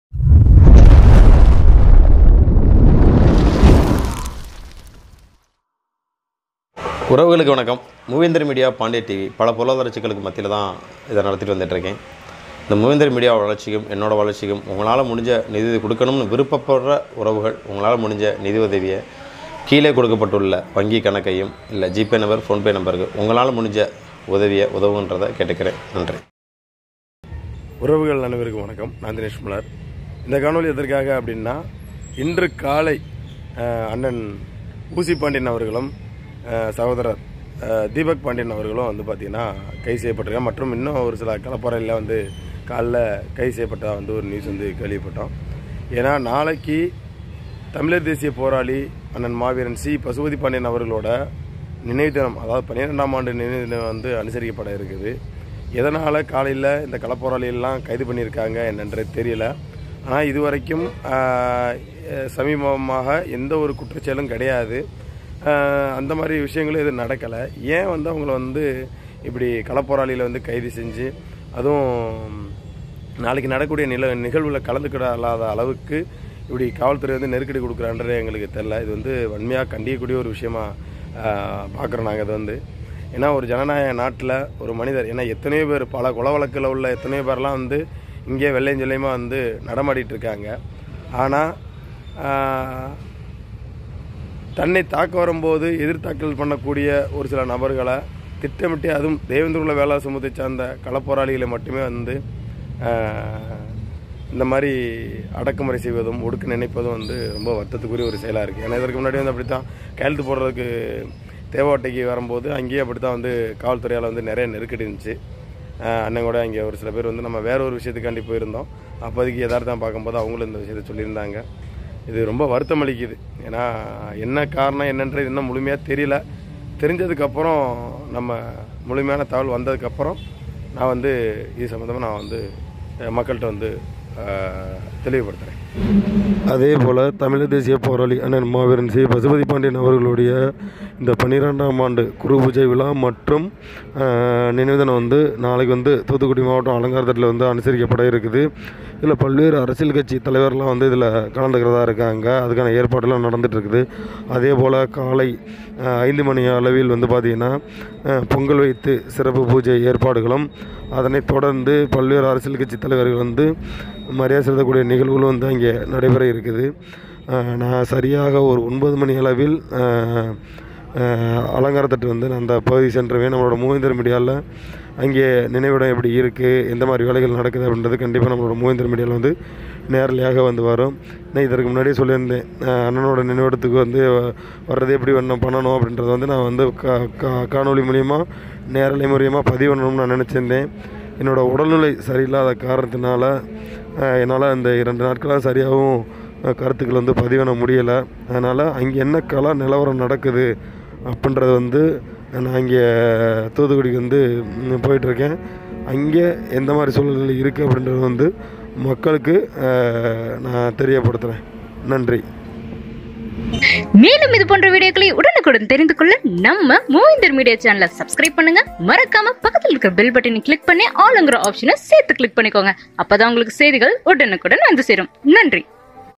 தீபக் பாண்டியன் ஊசி பாண்டியன் கைது
களப்போராளிகளை ஒடிக்கிறதா காவல்துறை
பாண்டியர்_TV லிங்க் 👇👇👇👇👇👇👇